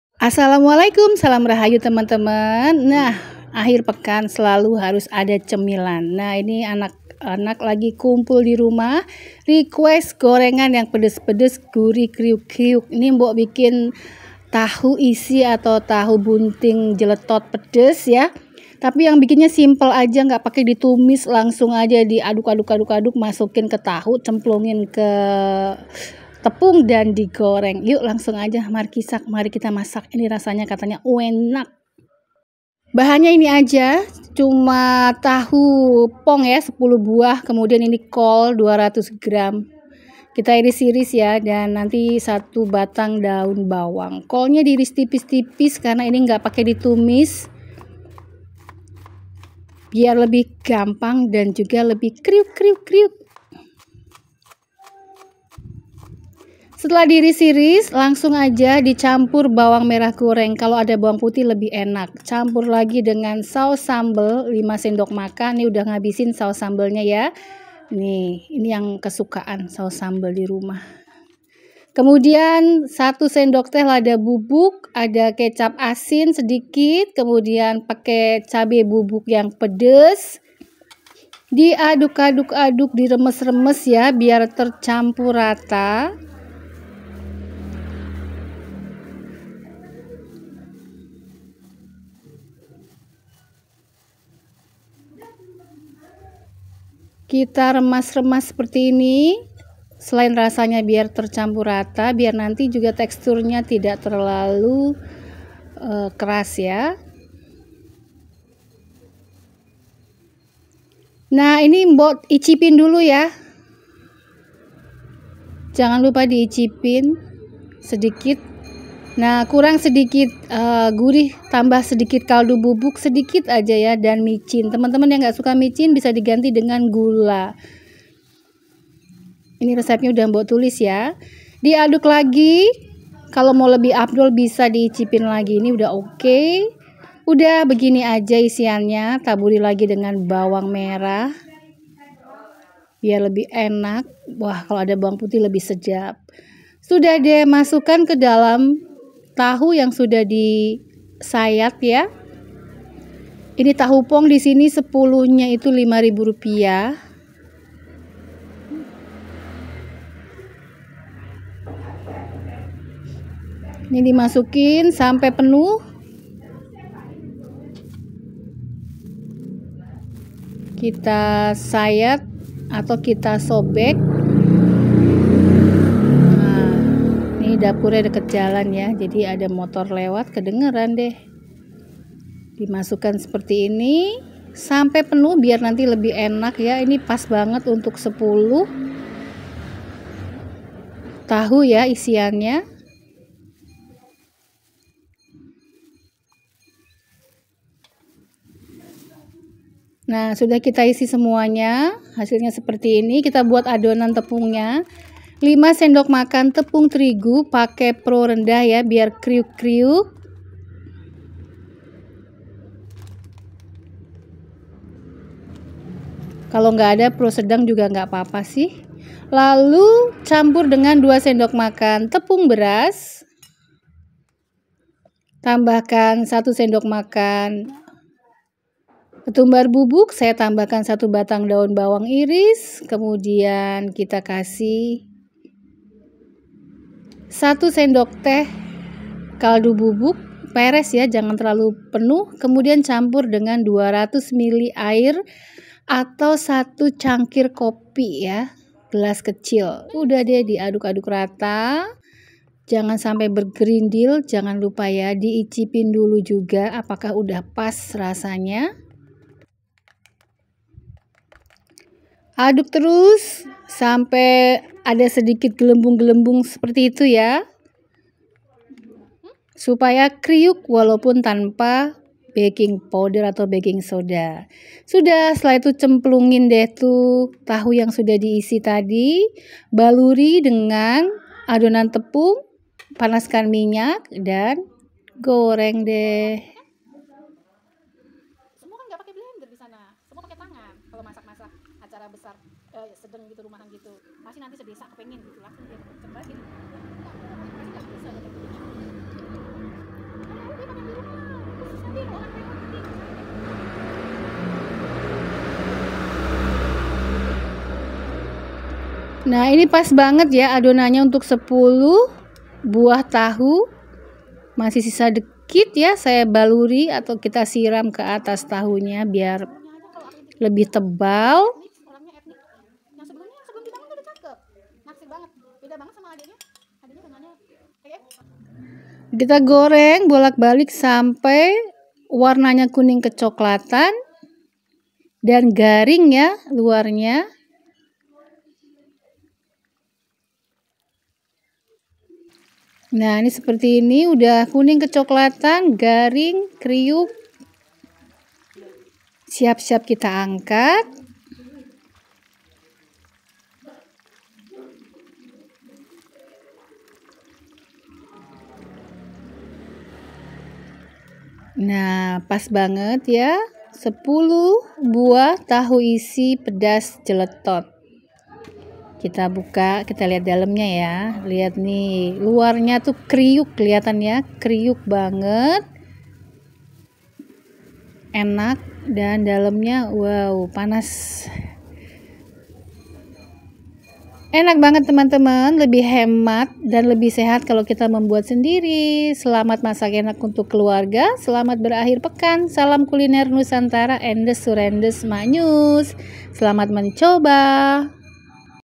Cuma 3 Bahan. Tidak Perlu Ditumis. Langsung Goreng
10 bh tahu pong
200 gr kol
2  bt daun bawang
2 sdm bawang merah goreng
5 sdm saos sambel
1 sdt cabai bubuk
1 sdt lada bubuk
1 sdt kecap asin
1/4 sdt kaldu bubuk
1/2 sdt msg/gula

adonan tepung:

5 sdm tepung trigu
2 sdm Tepung beras
200 ml air
1 sdt kaldu bubuk
1 sdm ketumbar bubuk
1 bt daun bawang

selamat mencoba 😍